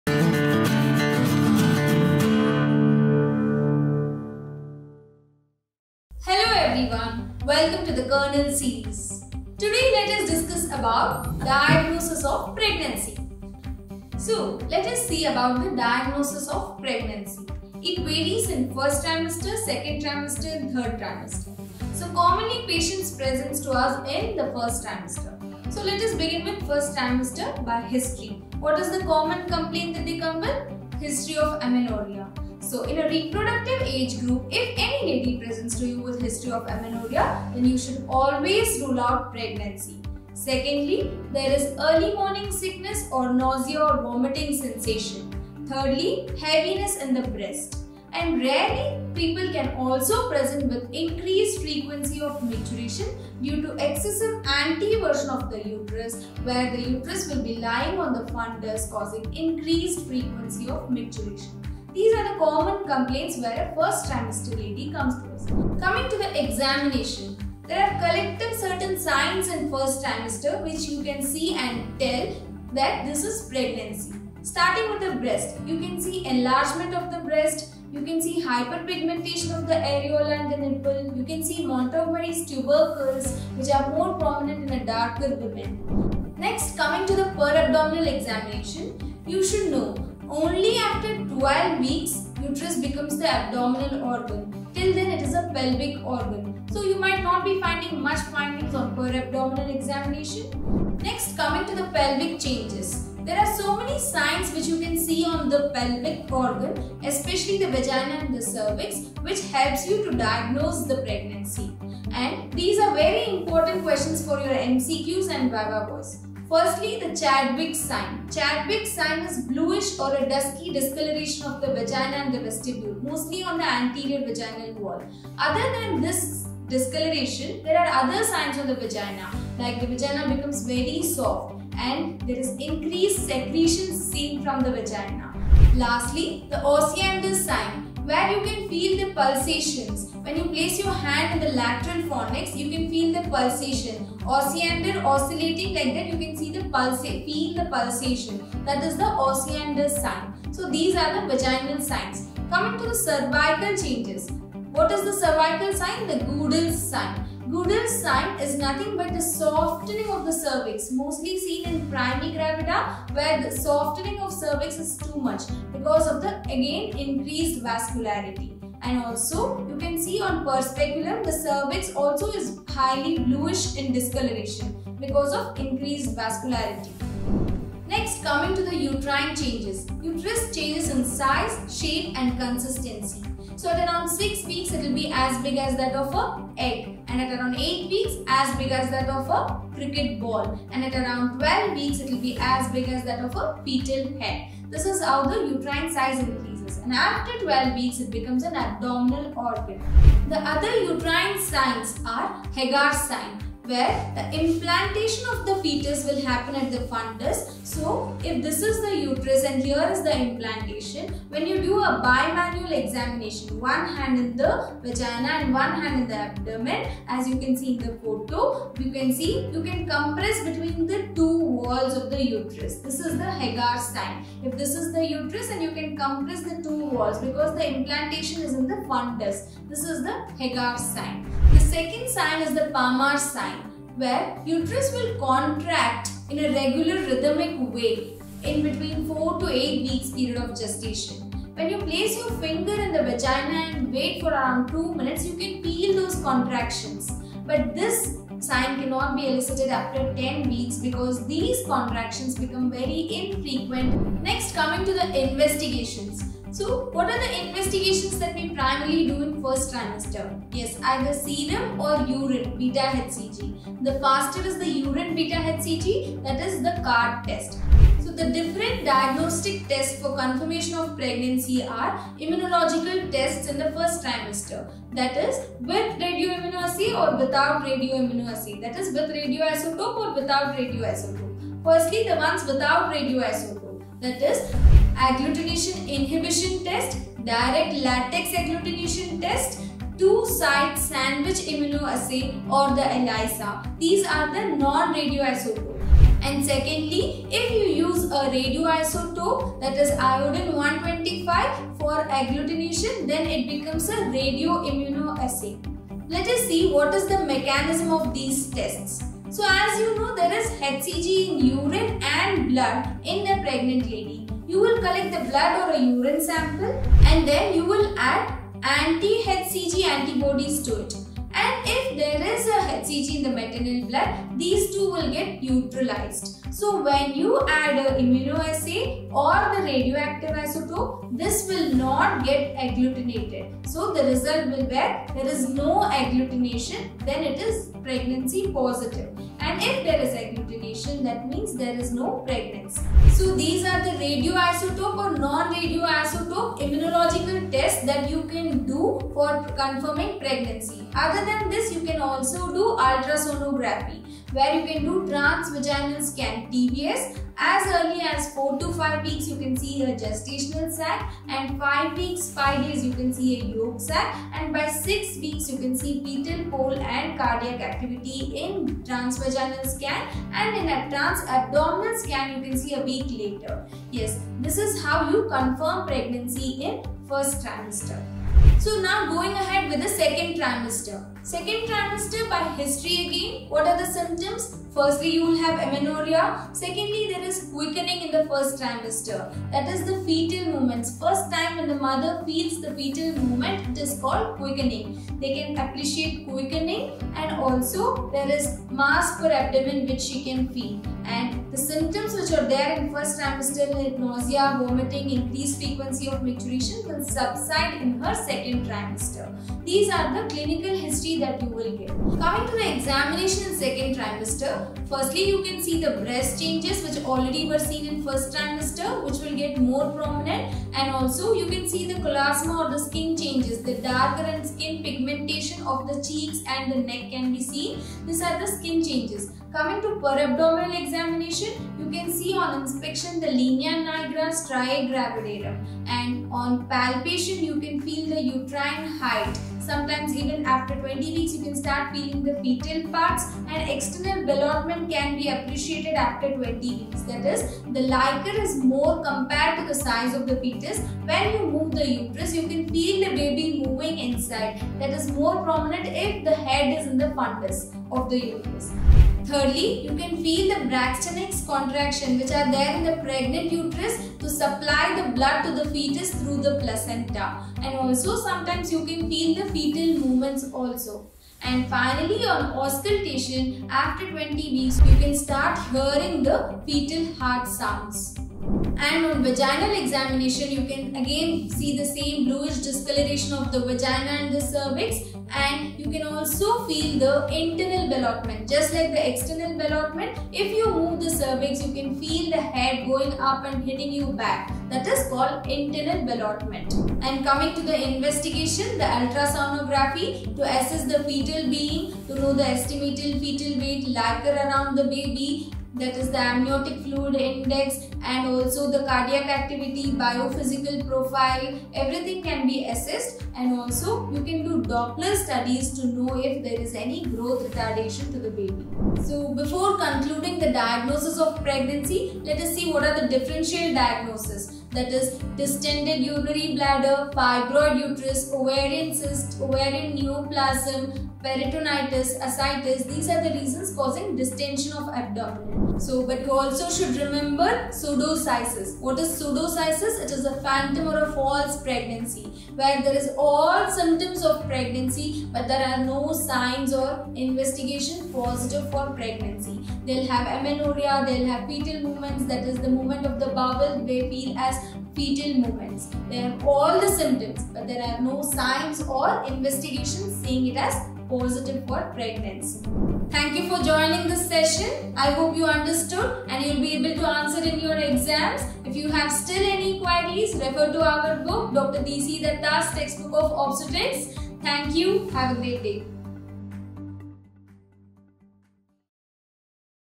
DIAGNOSIS OF PREGNENCY | DR. ROSHINI P
Lecture Series
Diagnosis of Pregnancy

Presenting you India’s FIRST SMART BOOK ON OBSTETRICS.
DC DUTTA’s TEXT BOOK OF OBSTETRICS
Hope everyone will enjoy THE KERNELSERIES.

For full video, refer DC DUTTA TEXT BOOK OF OBSTETRICS ; 10th EDITION